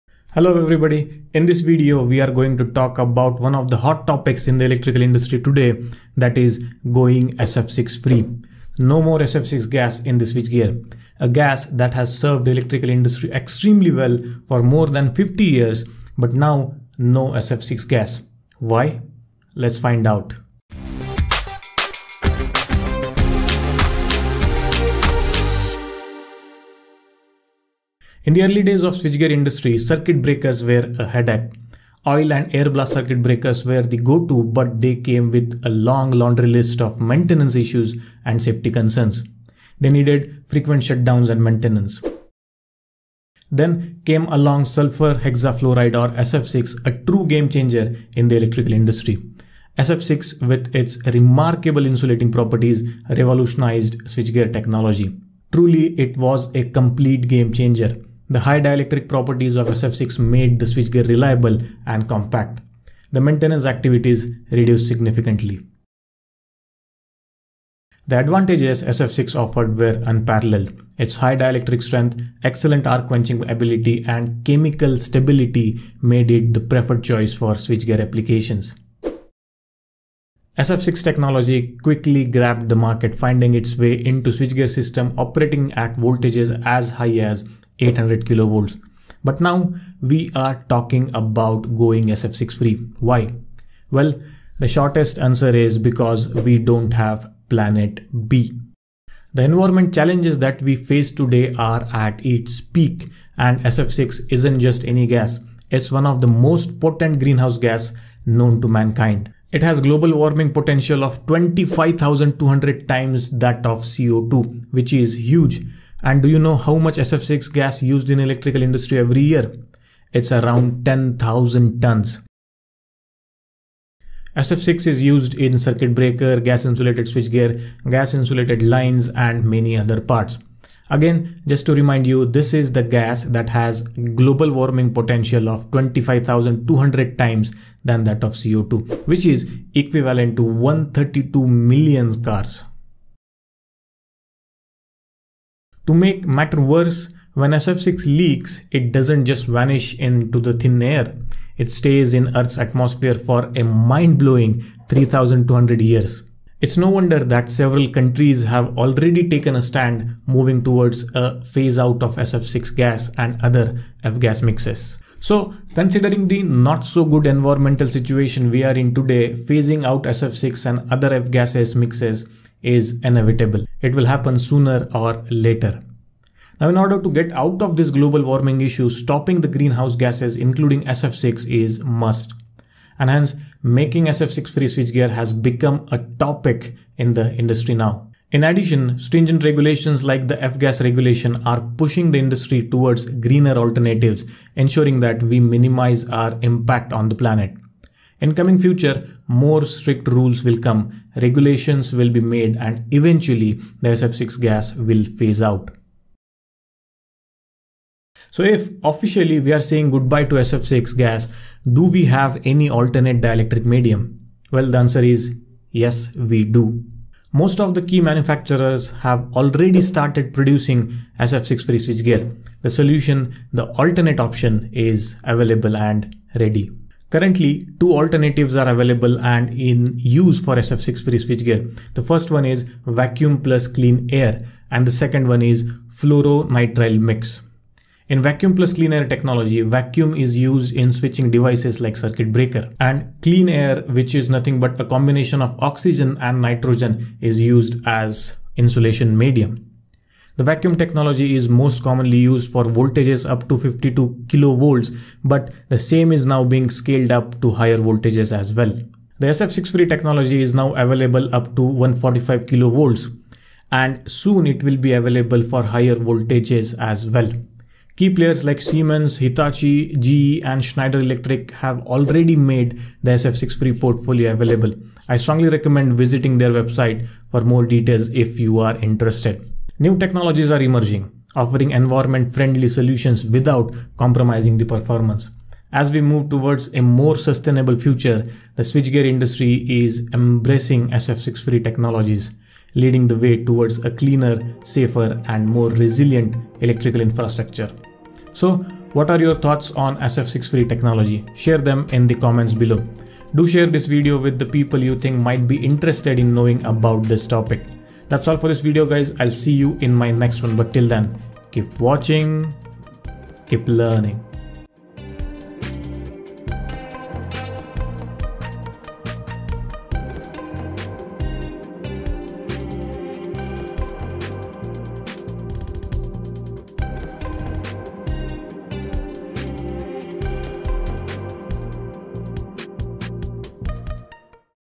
Goodbye SF6 l SF6-Free Switchgear l Why? TheElectricalGuy
SF6-Free Switchgear are getting developed. SF6 gas served electrical industry for more than 50 years. But now we are going SF6-Free. Why is that? Let's find it out with the help of this video.